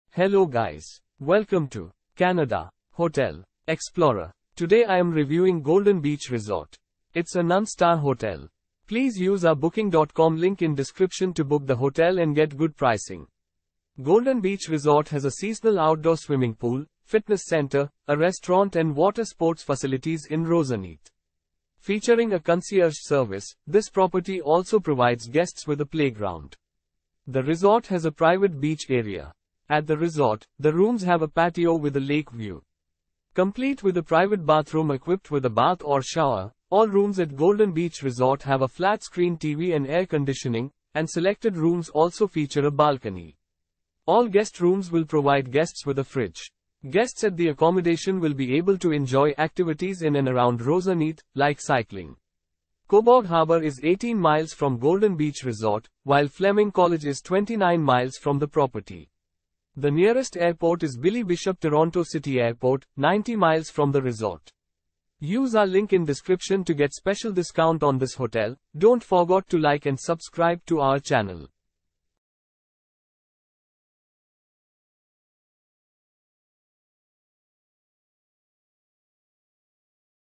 Golden Beach Resort Review - Roseneath , Canada
Golden Beach Resort

Golden Beach Resort has a seasonal outdoor swimming pool, fitness center, a restaurant and water sports facilities in Roseneath. Featuring a concierge service, this property also provides guests with a playground. The resort has a private beach area.

At the resort, the rooms have a patio with a lake view. Complete with a private bathroom equipped with a bath or shower, all rooms at Golden Beach Resort have a flat-screen TV and air conditioning, and selected rooms also feature a balcony. All guest rooms will provide guests with a fridge.

Guests at the accommodation will be able to enjoy activities in and around Roseneath, like cycling.

Cobourg Harbour is 18 miles from Golden Beach Resort, while Fleming College is 29 miles from the property. The nearest airport is Billy Bishop Toronto City Airport, 90 miles from the resort.